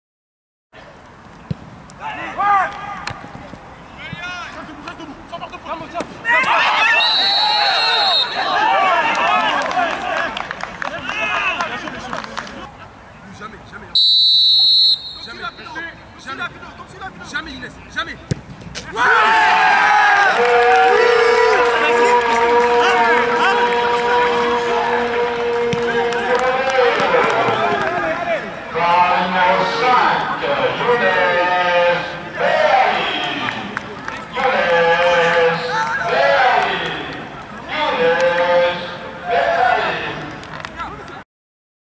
N3 : US Maubeuge-RC Lens 1-0
L'US Maubeuge a reçu  l'équipe réserve du RC Lens au stade Léo-Lagrange le samedi 22 octobre 2022. Victoire 1-0 de l'USM avec un penalty de Younes Bellali.